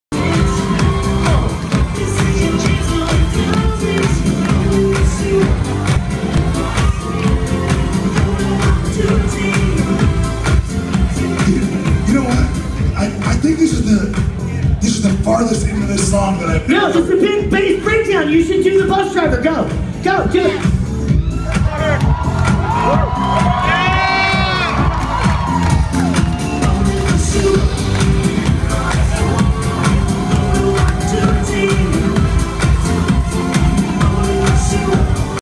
Bowling For Soup 20th Anniversary - Chris Burney does the bus driver
BFS 20th Anniversary show at House of Blues Dallas.  Chris Burney does the bus driver.